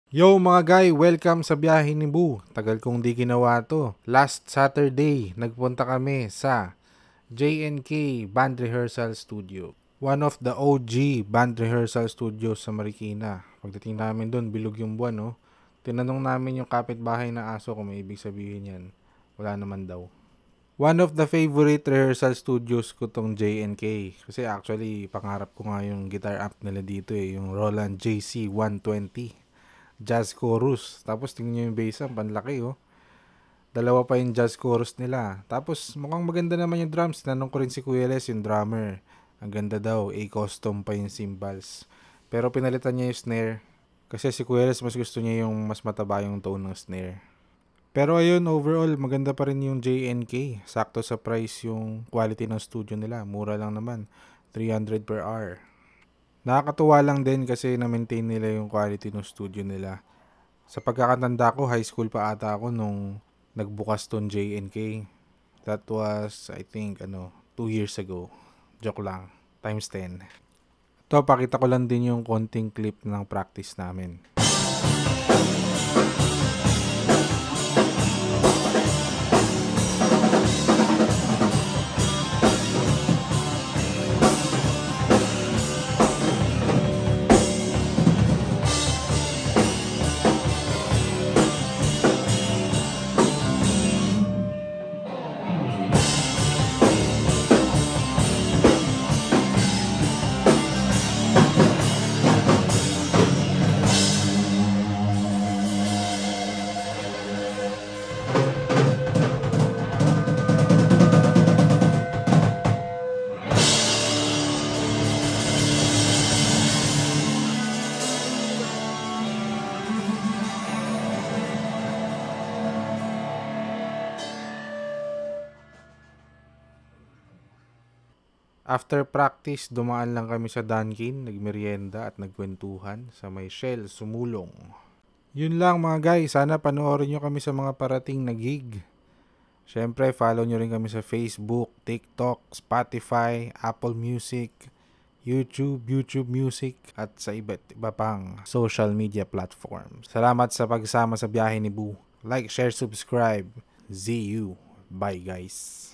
One of the OG band rehearsal studios sa Marikina
Nag ensayo ang The Boo sa one of the OG band rehearsal studios sa Marikina. Samahan niyo kami sa J&K!